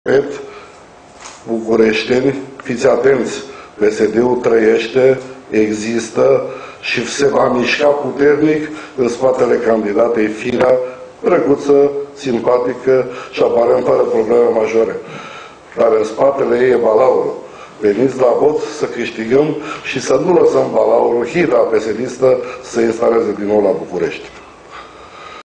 Conferință de presă susținută de prim-vicepreședintele PNL, Teodor Atanasiu, și deputatul PNL, Gh...
Teodor Atanasiu: Veniți la vot să câștigăm și să nu lăsăm hiena PSD-istă să se instaleze din nou la București!
Titlul evenimentului: Conferință de presă susținută de prim-vicepreședintele PNL, Teodor Atanasiu, și deputatul PNL, Gheorghe Ialomițianu